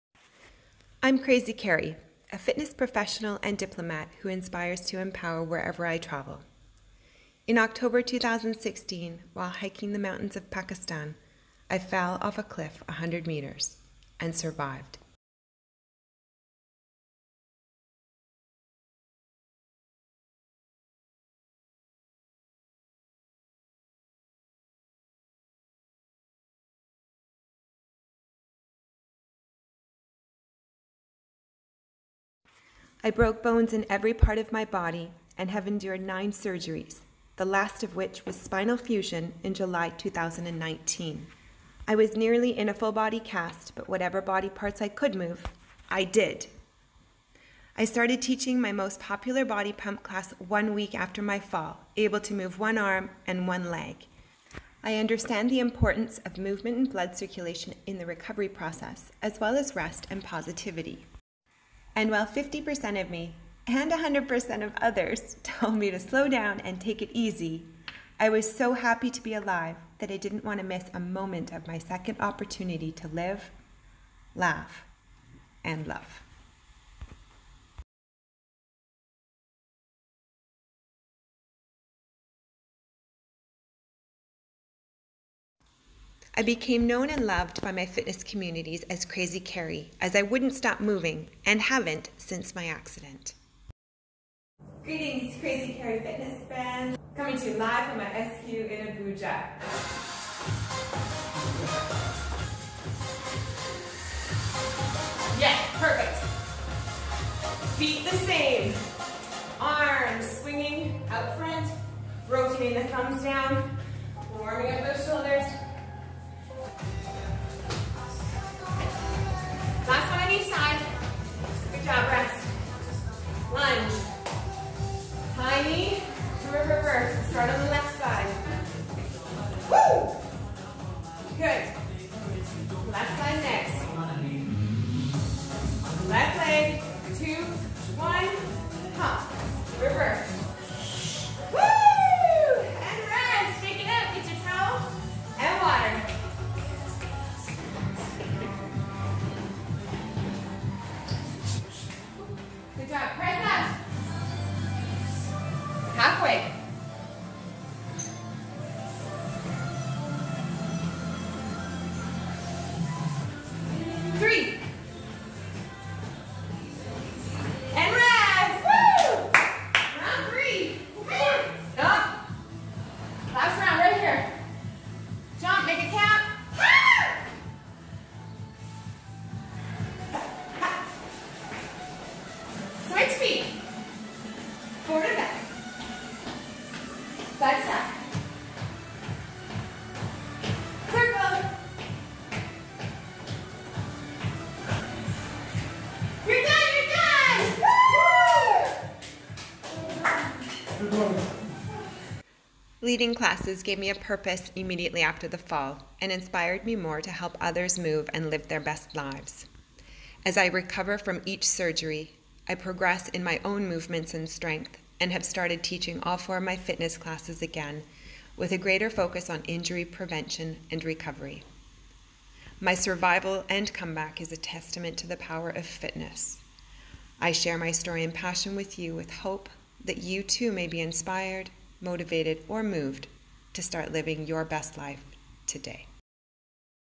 Crazy Kerri - Get to know her!
I’m Crazy Kerri, a Fitness Pro and Diplomat who inspires to empower wherever I travel. A few years ago, while hiking the mountains of Pakistan, I fell off a cliff 100 meters and survived! I broke bones in every part of my body and endured 9 surgeries, including back fusion. It's a MIRACLE to be alive!!  My survival and comeback is a testament to the power of fitness, that has kept me strong physically and emotionally.

I've always been sporty and bubbly, but my accident galvanized my calling to keep others moving towards their full fitness potential. This 3rd fall and 2 previous ones, have given me experience and knowledge with injury prevention and recovery that cannot be learned elsewhere! I bring HIIT, Tabata, Body Pump, Aquafit, and Yoga into my programming and take personal limitations seriously. My magic is my insane amount of energy, my exuberant spirit, and a commitment to the well-being of others, connected to a global fitness community.